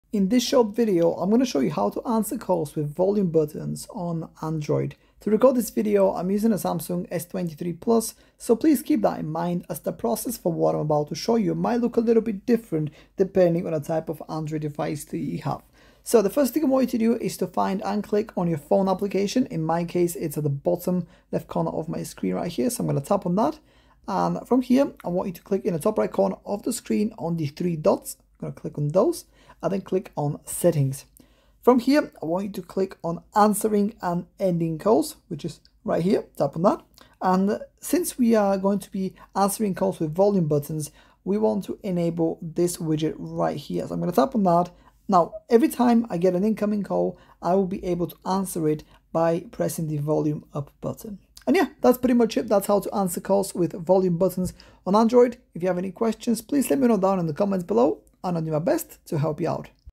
How to Answer Calls With Volume Buttons on Android [QUICK GUIDE]
In this video, I'm going to show you how to answer calls with volume buttons on Android.